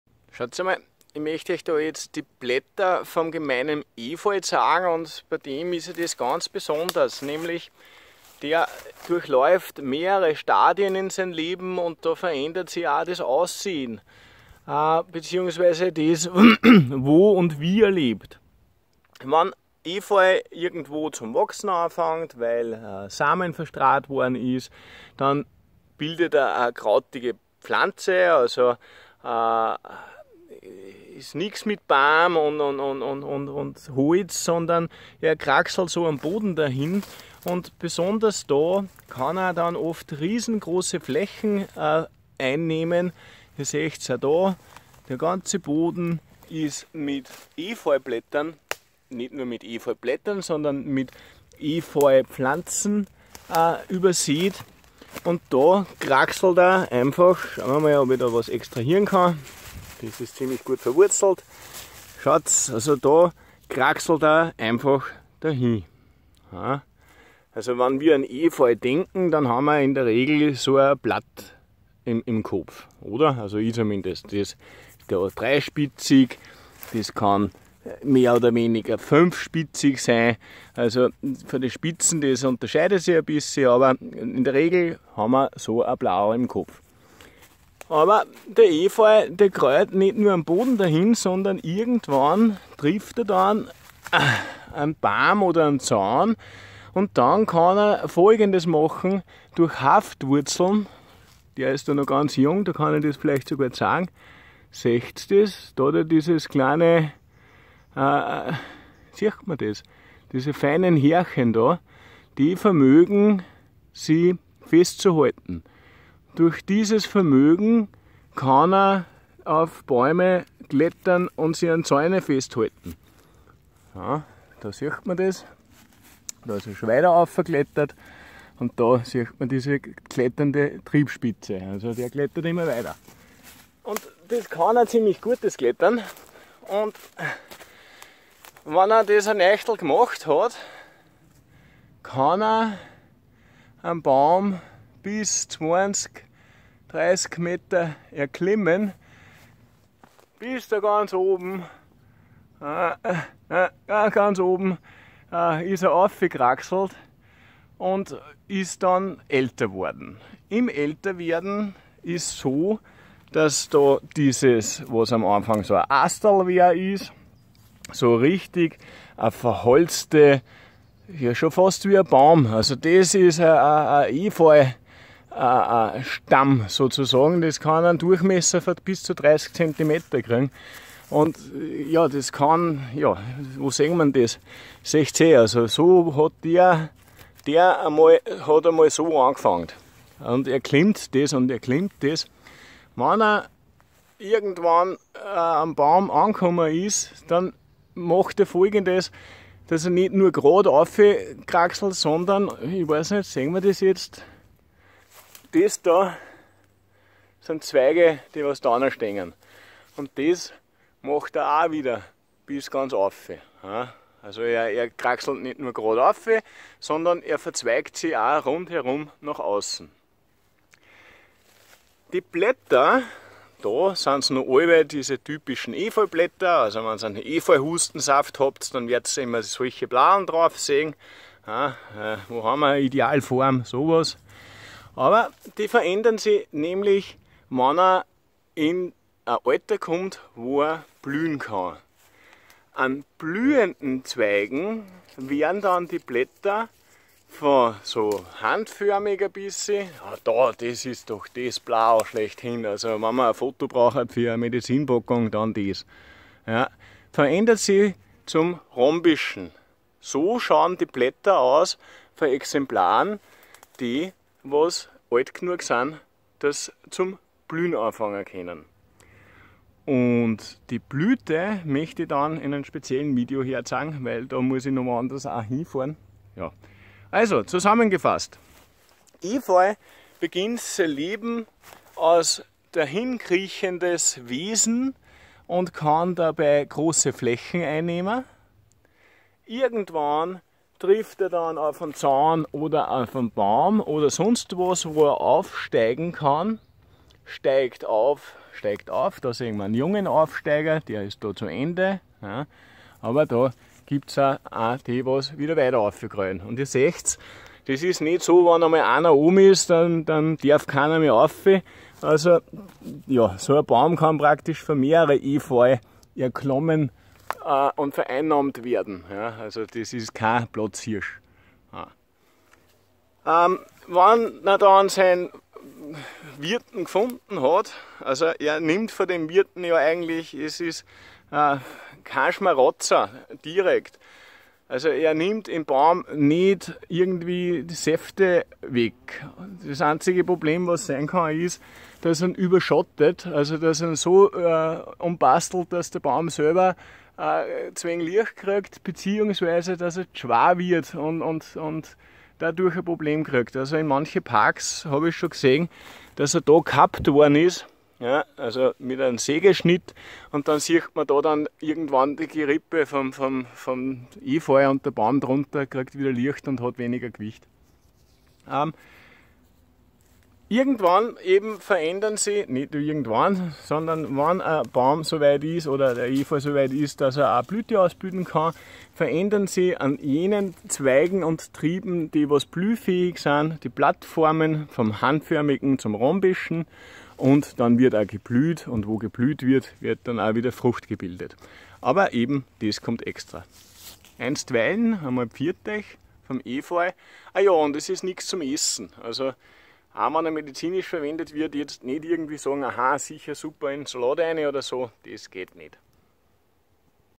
Gemeiner Efeu - Blatt/Blätter - 20.09.18 (Hedera helix) - giftige Wildpflanzen bestimmen
Der Gemeine Efeu, auch Gewöhnlicher Efeu oder kurz Efeu (Hedera helix) genannt. Gezeigt werden die vielgestaltigen, je nach Entwicklungsstation anders erscheinenden, Blätter. Es werden Eigenschaften und Merkmale erklärt.

DANKE für dein DANKE und deine UNTERSTÜTZUNG!